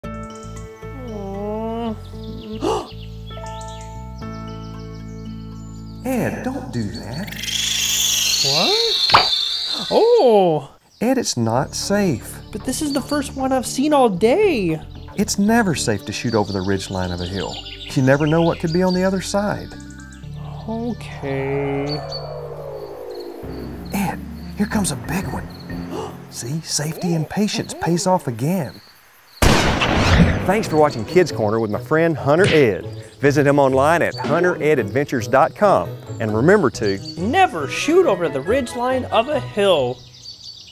Kids Korner with Jimmy Sites - Never Shoot over the Ridgeline of a Hill
Jimmy Sites and Hunter Ed share another important safety tip on Kids Korner.